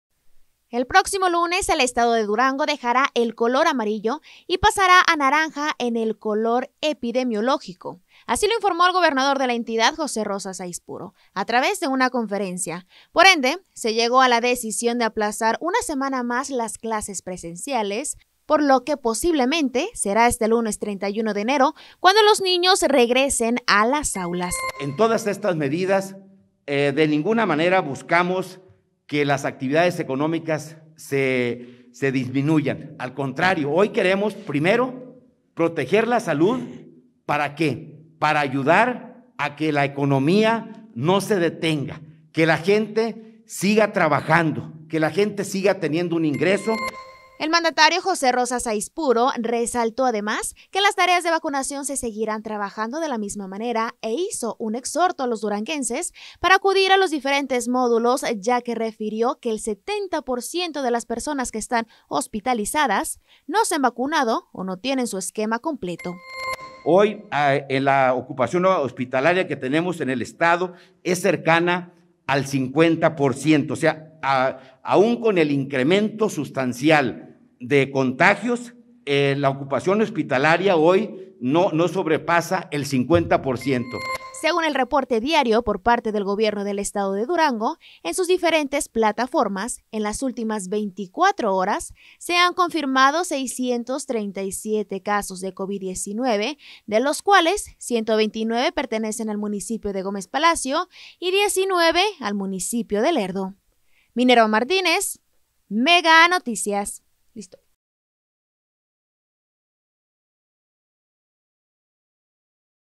Durango va a semáforo naranja: clases presenciales se aplazan.
→ El estado de Durango pasará a semáforo naranja en el color epidemiológico debido al incremento de casos COVID. Entre sus medidas, se tomó la decisión de aplazar una semana más las clases presenciales.